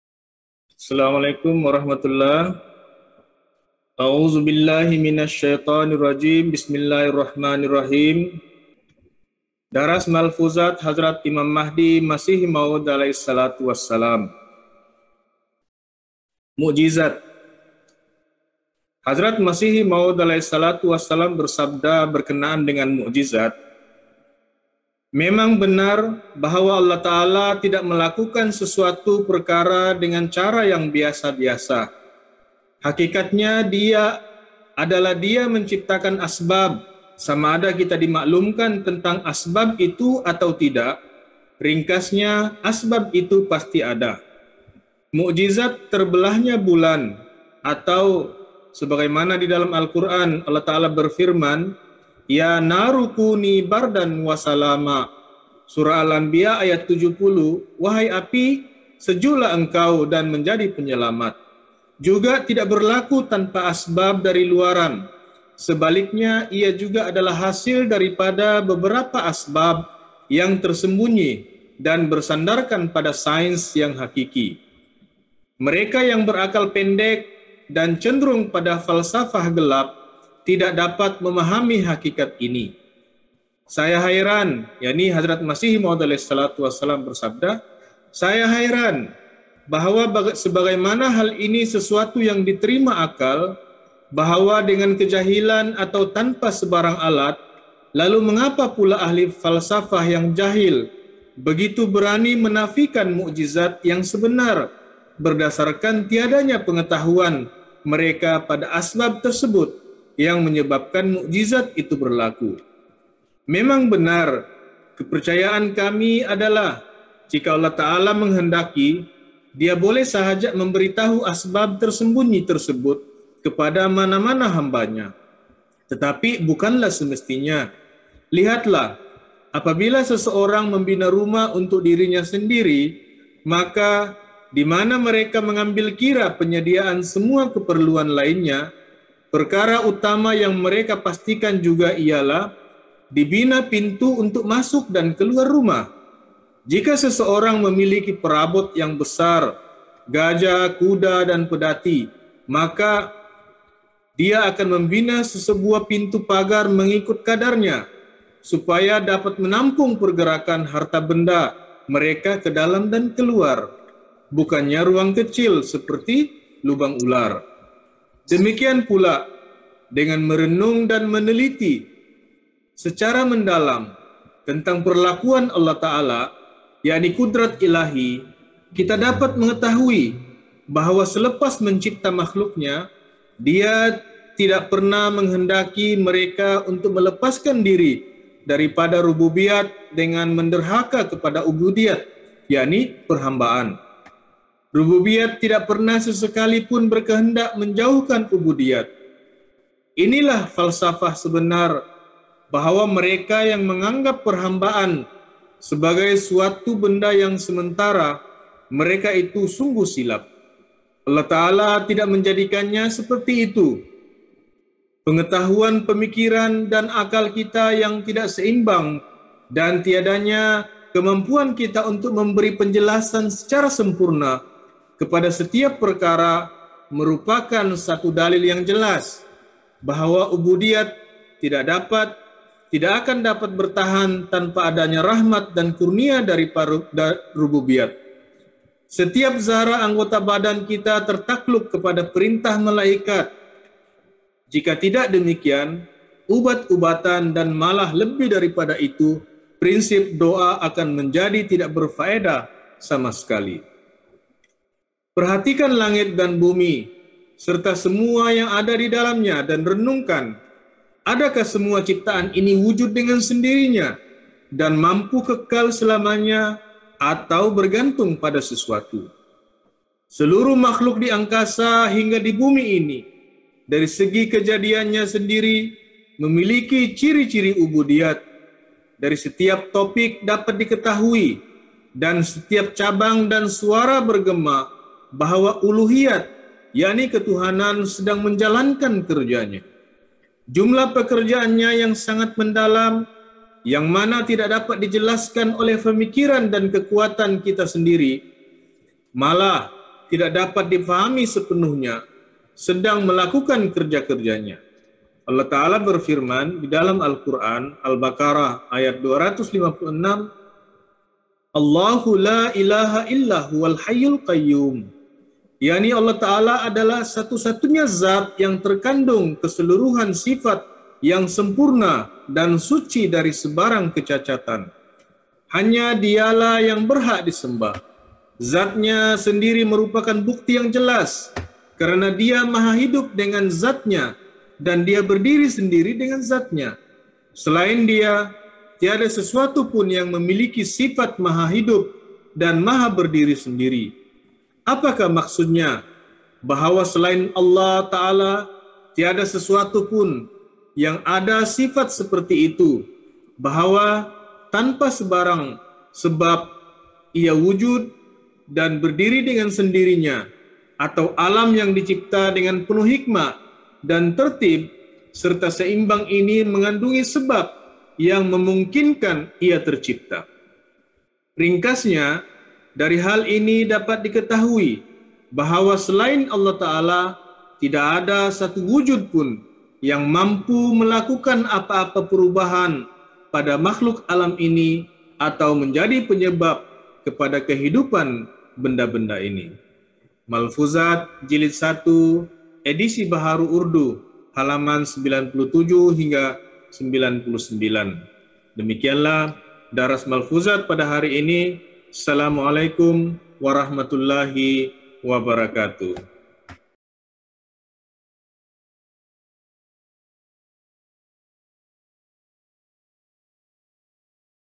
Daras Malfuzat (39) | 09.11.2021
Rakaman Daras Maghrib dari buku Malfuzat (Kumpulan sabda-sabda Hazrat Masih Mau'ud alaihissalam)

Malfuzat, Jilid 1 halaman 97 - 99, disampaikan oleh Mln. Salahuddin Syed Ali